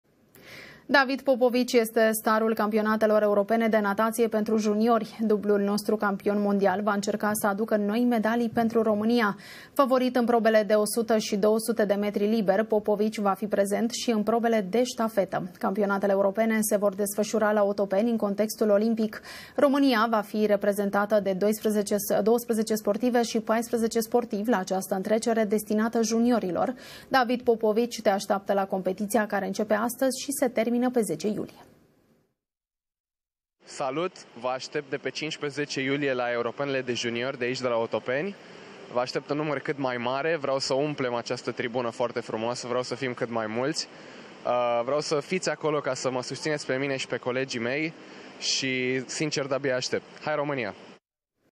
David Popovici, dublul nostru campion mondial, vrea să aducă noi medalii pentru România
➥ David Popovici va fi la startul campionatului european de înot pentru juniori.
➥ Concursul se va desfășura la bazinul din Otopeni, de pe 5 până pe 10 iulie.
➥ România are 26 de sportivi angrenați în probele europeanului de natație.
➥ David Popovici va concura în probele de 50, 100 și 200m liber.